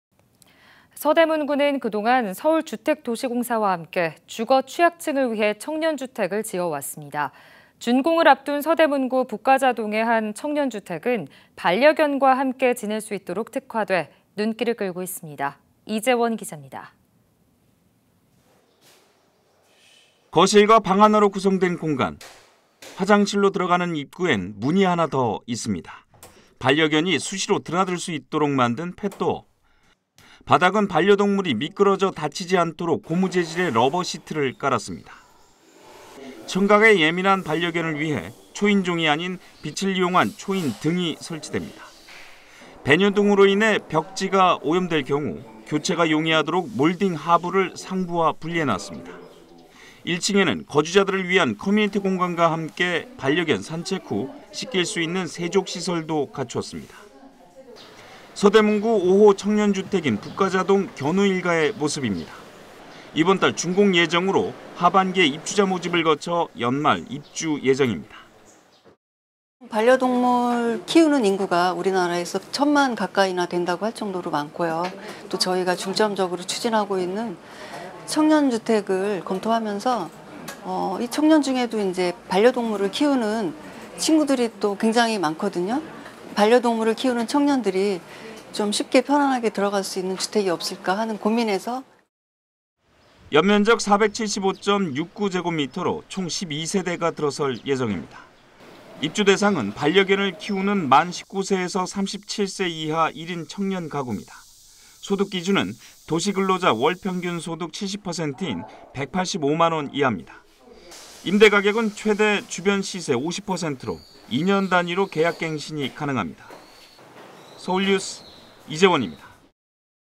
‘반려견과 동거하는 청년주택’...임대료 시세 50%_SK broadband 서울뉴스
[서울뉴스 이재원기자]

[앵커멘트]
서대문구는 그동안 서울주택도시공사와 함께
주거 취약층을 위해 청년주택을 지어왔습니다.
준공을 앞둔 서대문구 북가좌동의 한 청년주택은
반려견과 함께 지낼 수 있도록 특화돼
눈길을 끌고 있습니다.
이재원 기자입니다.

[기사내용]
거실과 방 하나로 구성된 공간.

화장실로 들어가는 입구엔
문이 하나 더 있습니다.

반려견이 수시로 드나들 수 있도록 만든 펫도어.

바닥은 반려동물이 미끄러져 다치지 않도록
고무재질의 러버시트를 깔았습니다.

청각에 예민한 반려견을 위해
초인종이 아닌 빛을 이용한 초인등이 설치됩니다.

반려견의 배뇨 등으로 인해 벽지가 오염될 경우
교체가 용이하도록 몰딩 하부를
상부와 분리해 놓았습니다.

1층에는 거주자들을 위한 커뮤니티 공간과 함께
반려견 산책 후 씻길 수 있는
세족시설도 갖추었습니다,

서대문구 5호 청년주택인
북가좌동 견우일가의 모습입니다.

이번 달 준공 예정으로
하반기에 입주자 모집을 거쳐
연말 입주 예정입니다.

이나령 서대문구 사회복지과장
(반려동물 인구가 증가하고 있는 가운데
2~30대 주거취약계층도
반려견과 생활할 수 있는...)

연면적 475.69㎡로
총 12세대가 들어올 예정입니다.

입주 대상은 반려견을 키우는
만 19세에서 37세 이하 1인 청년 가굽니다.

소득 기준은 도시근로자 월평균 소득 70%인
185만 원 이합니다.

임대 가격은 최대 주변 시세 50%로
2년 단위로 계약 갱신이 가능합니다.

서울뉴스 이재원입니다.